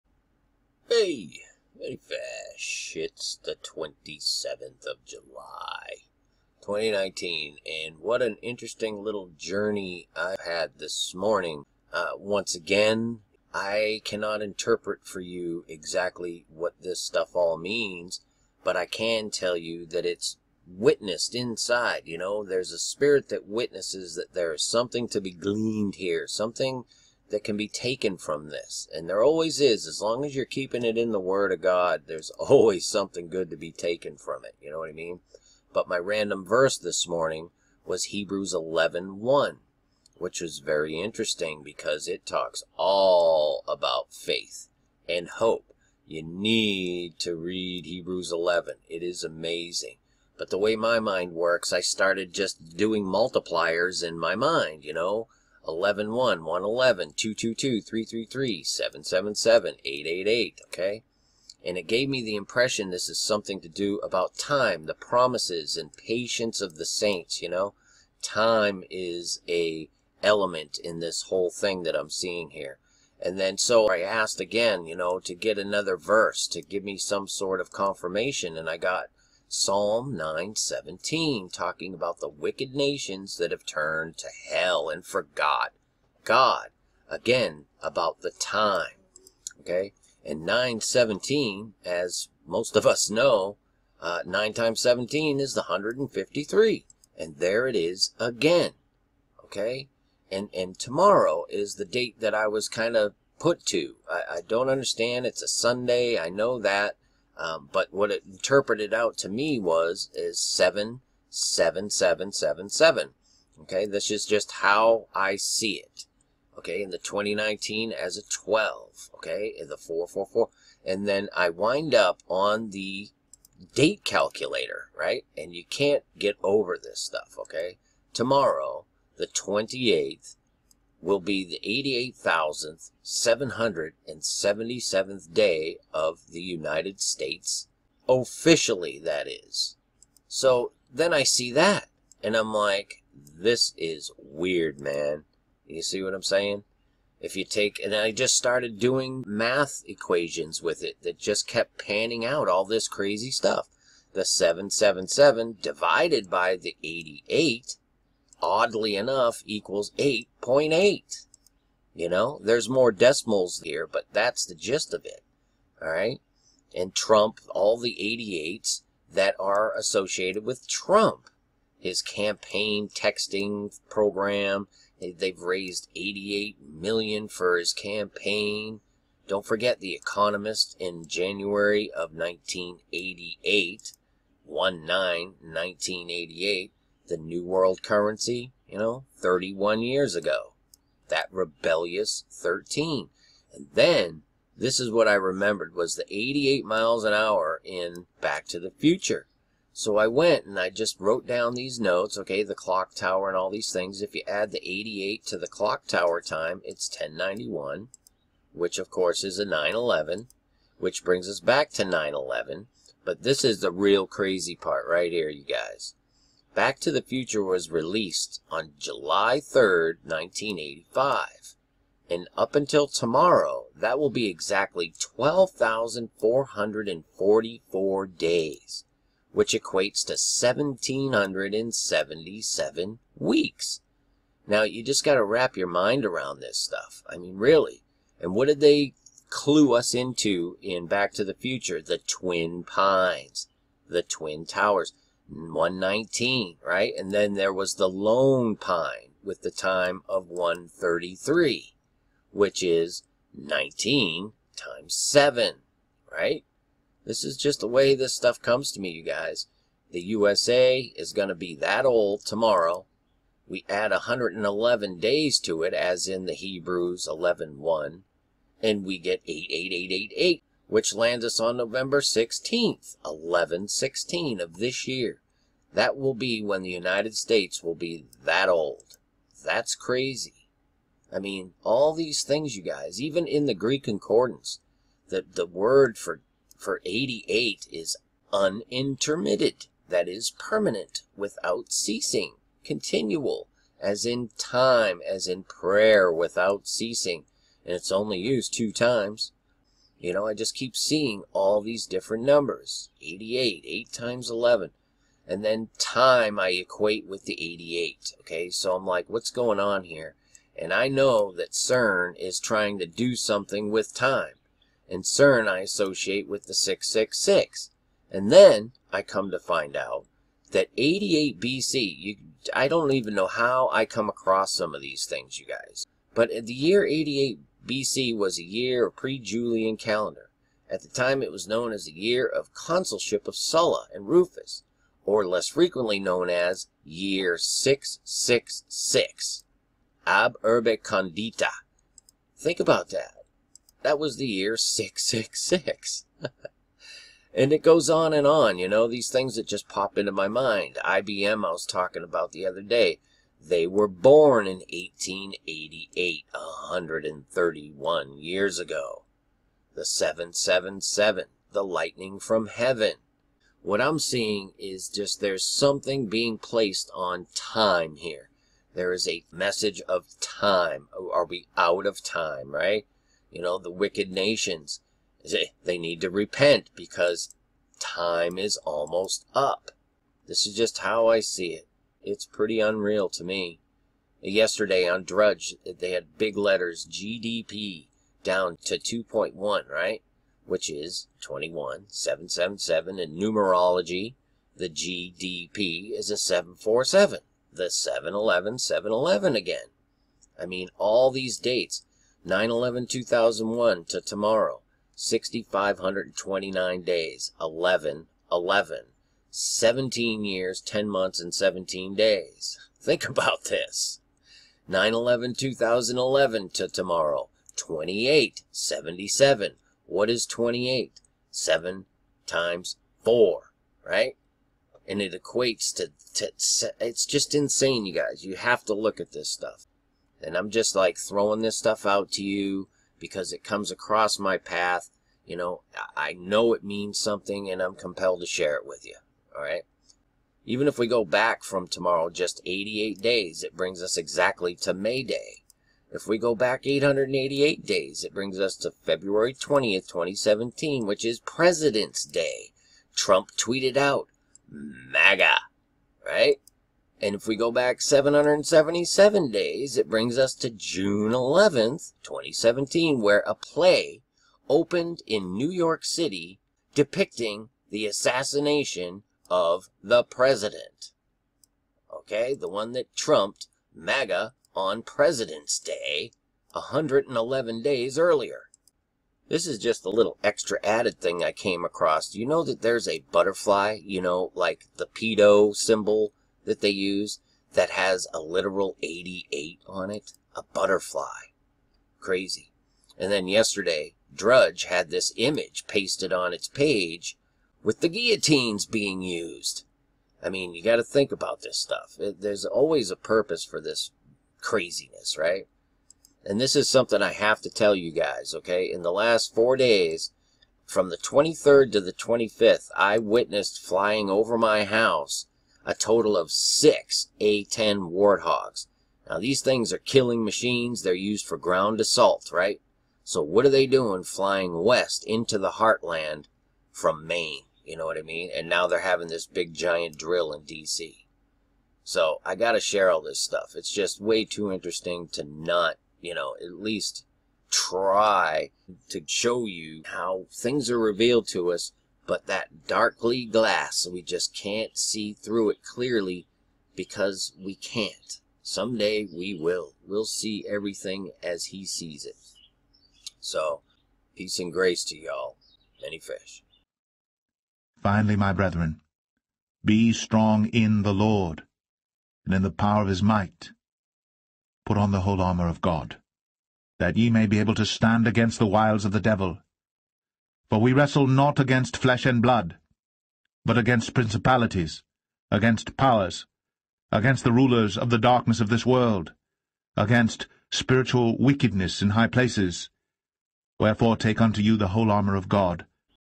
7/28 USA 88,777 Back to the Future. Time is Short. Heads Up.
These  A-10s buzzin around up here is highly out of ordinary. Add this secret surprise mil drill in DC = heads up! All these things mean something, but the most valuable thing we have is our relationship with our one true Lord and Savior Jesus Christ! No fear in Him! His peace and grace to you all!

Amen.